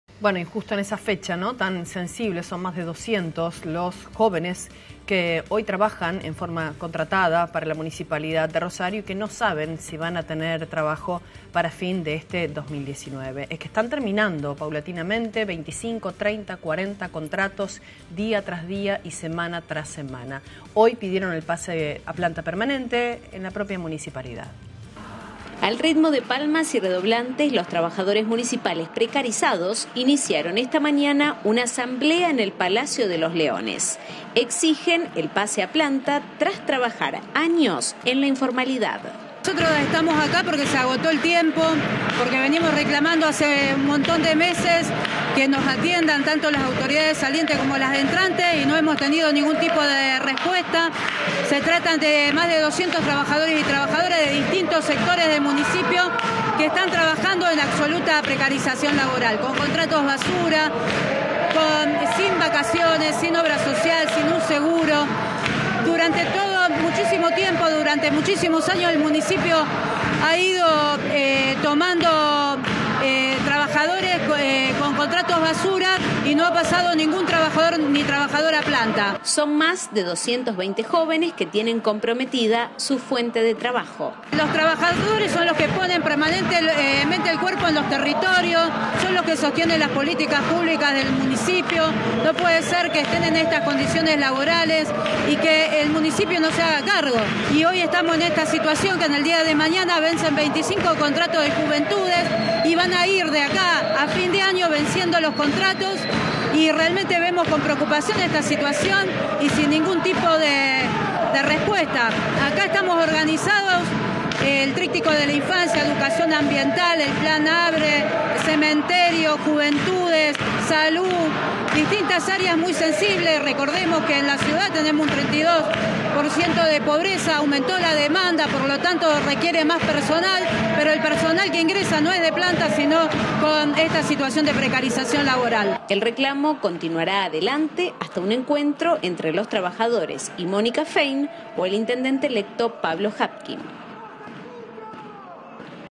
Trabajadores municipales exigen el pase a planta permanente-Telefe Rosario
Son 220 contratos que finalizan hacia fin de año.